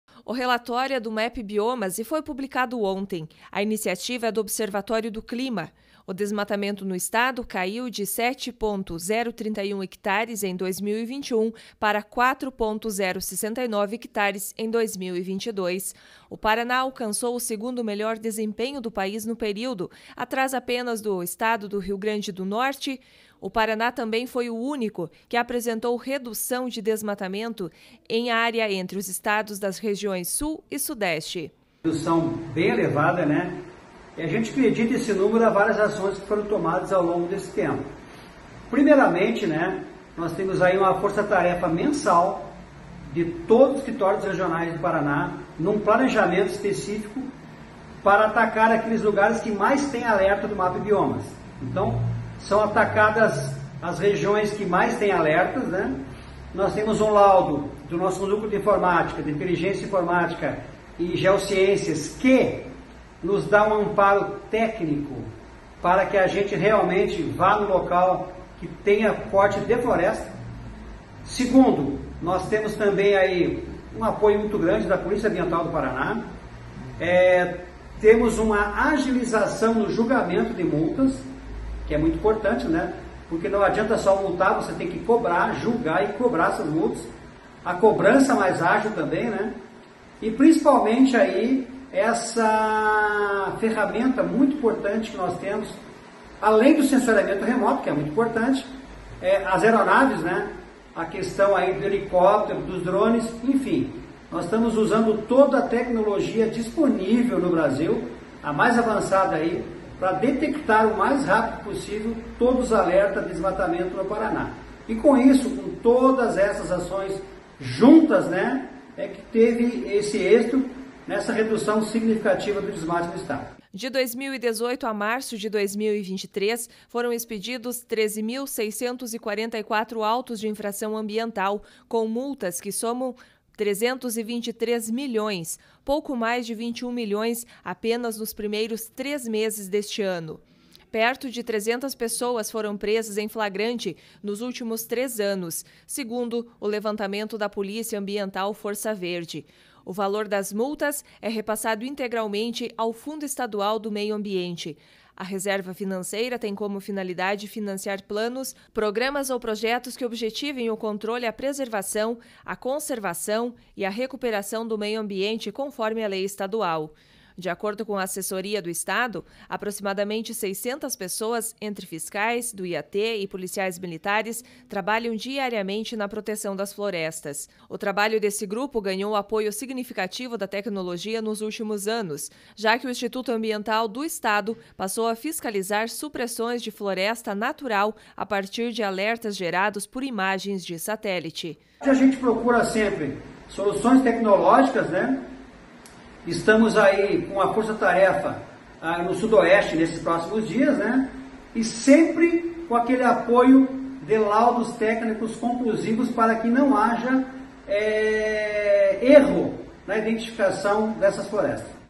Desmatamento reduziu 42% no Paraná em 2022
Meio Ambiente: O desmatamento diminuiu 42% entre 2021 e 2022 no Paraná. O estado alcançou o segundo melhor desempenho do País no período.